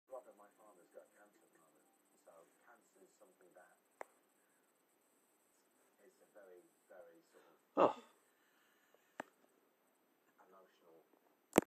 Fly doing the wall of death in a lampshade :)
This Fly got stuck in the lampshade and couldn't get out, it was doing the wall of death for ages before I started filming it.